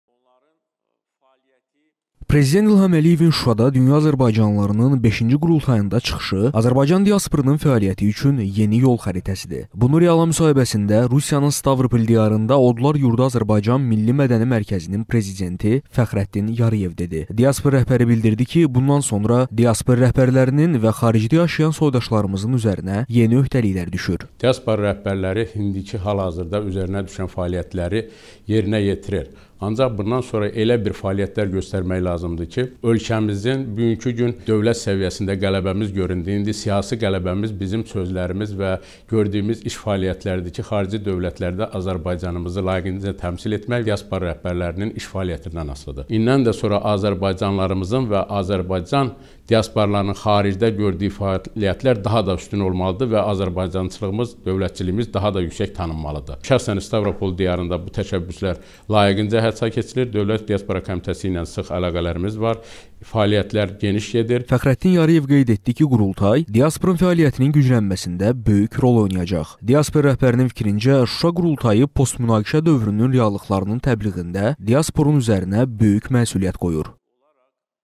Diasporun yeni fəaliyyət istiqamətləri
F.Yarıyev: Şuşa qurultayında yeni "yol xəritəsi" müəyyənləşdi